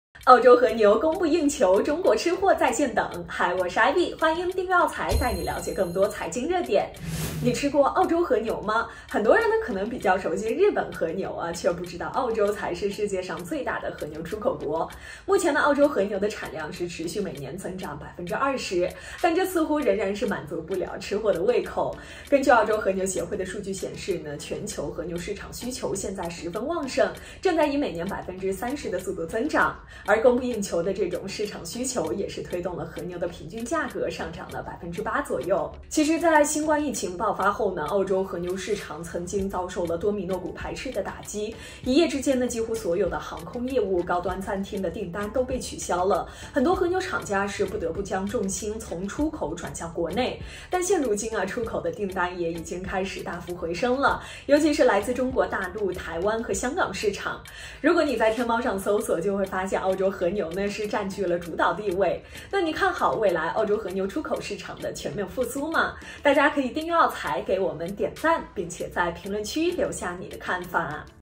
澳洲和牛供不应求！中国吃货“在线等”！
澳洲和牛供不应求！中国吃货“在线等”！根据澳洲和牛协会的数据显示，目前全球市场需求正在以每年30%的速度增长，而和牛年产量的增长率只有20%。因此推动了和牛平均价格上涨了8%。在新冠疫情爆发后，澳洲和牛市场曾遭受断崖式冲击，一夜之间几乎所有航空公司、高端餐厅的订单都被取消，很多和牛厂家不得不将业务重心从出口转向澳洲国内。但现如今，出口订单已经大幅回升，尤其是来自中国大陆、台湾和香港市场。如果在天猫上搜索，就发现澳洲和牛占据了主导地位。那你看好未来澳洲和牛出口市场的全面复苏吗？大家可以订阅@澳财 ，给我们点赞、留言分享吧！